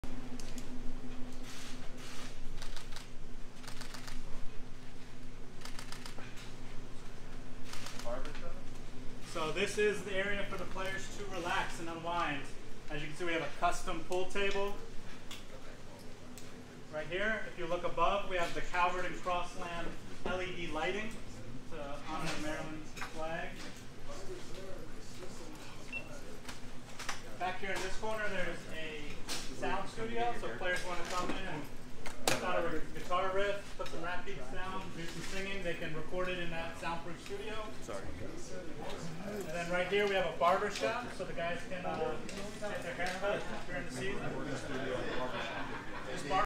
Jones-Hill House Player Lounge
A first look at Maryland football’s new players lounge, which includes a pool table, music studio and barber shop.